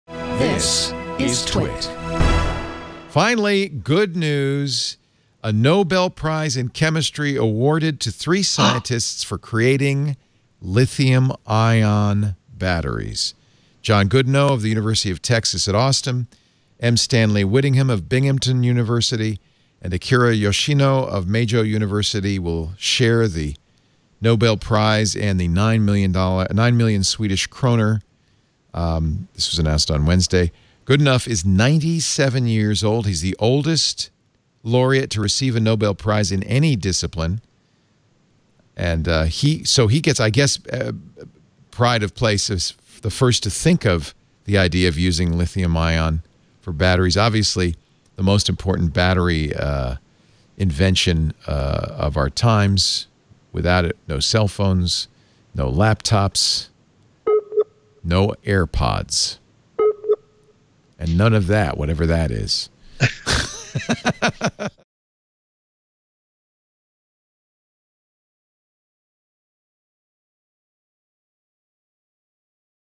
Lithium-ion inventors win Nobel Prize
Lithium-ion inventors win a well-deserved Nobel Prize.